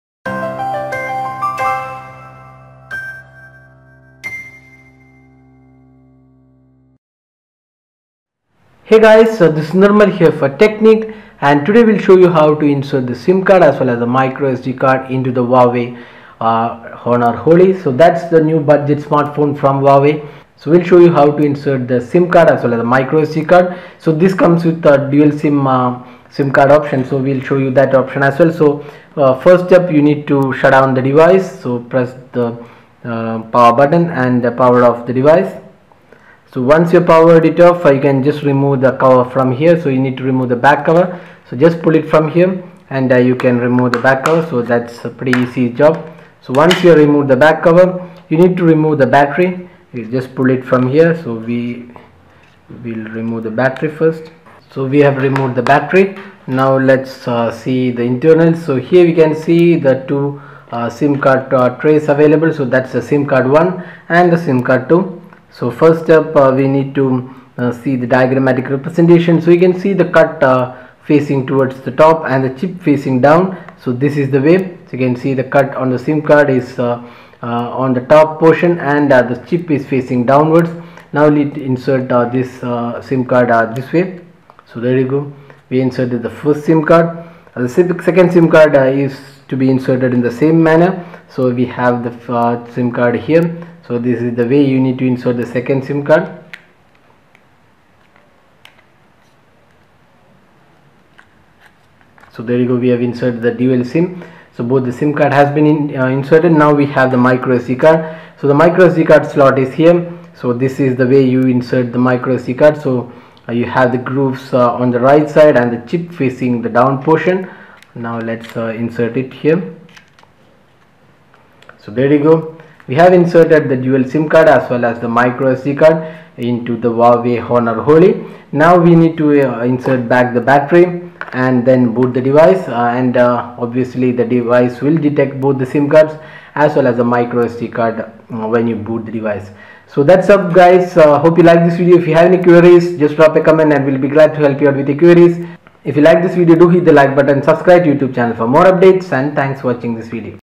How to Insert SIM card and microSD card into Huawei Honor Holly
Huawei Honor Holly comes with dual SIM card and also support for micro SD card, here is how to insert the SIM cards and also the SD card into the device.